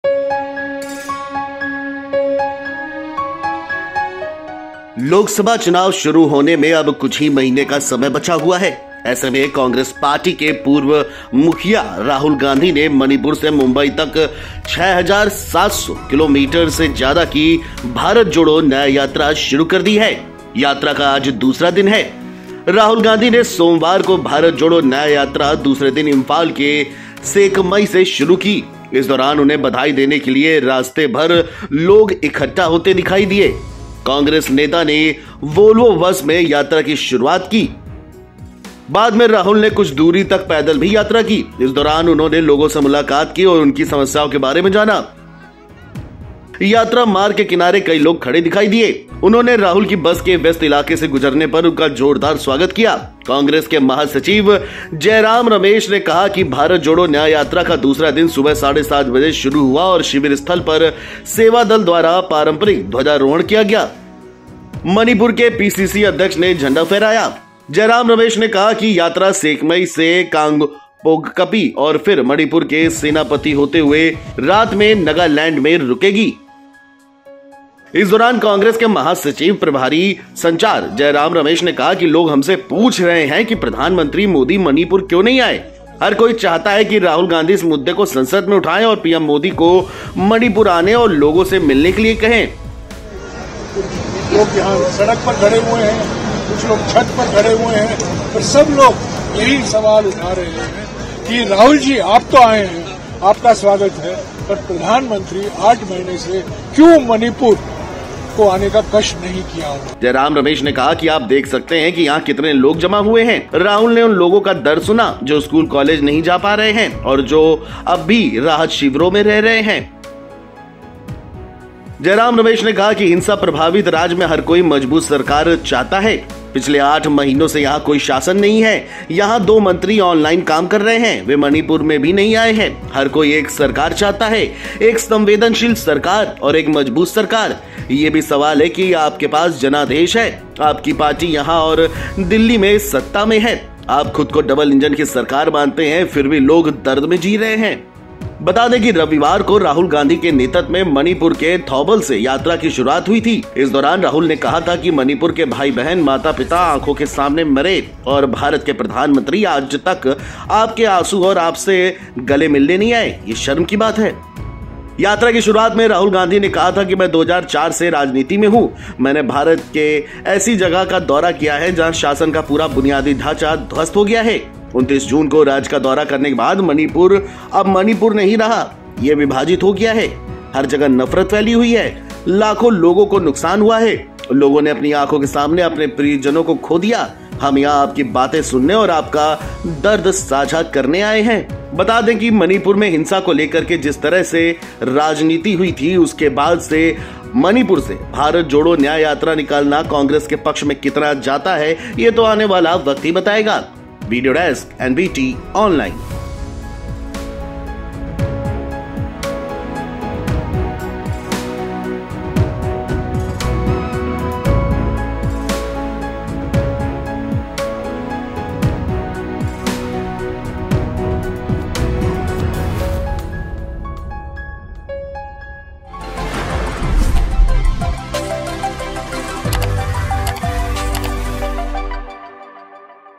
Bharat Jodo Nyay Yatra: Rahul Gandhi की न्याय यात्रा में क्यों हुआ PM Modi का जिक्र | NBT
लोकसभा चुनाव शुरू होने में कुछ ही महीने बचे हैं। ऐसे में कांग्रेस पार्टी के पूर्व प्रमुख राहुल गांधी ने मणिपुर से मुंबई तक 6,700 किलोमीटर से ज्यादा की भारत जोड़ो न्याय यात्रा शुरू कर दी है। आज उनकी यात्रा का दूसरा दिन है। इस दौरान उन्हें बधाई देने के लिए रास्ते पर लोग जमा दिखाई दिए।